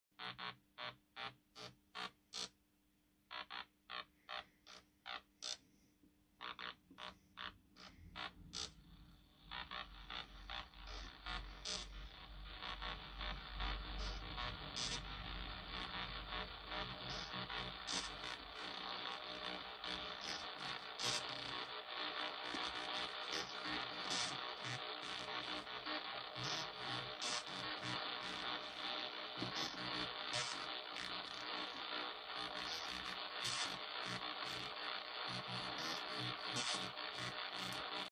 Modular: Sequencer DSP Chaos
Adding major chaos to a simple sequence with the Black Hole DSP.

E340 Cloud Generator sequenced by Analalogue Solutions Oberkorn through Intellijel Korgasmatron and WMD Dual VCA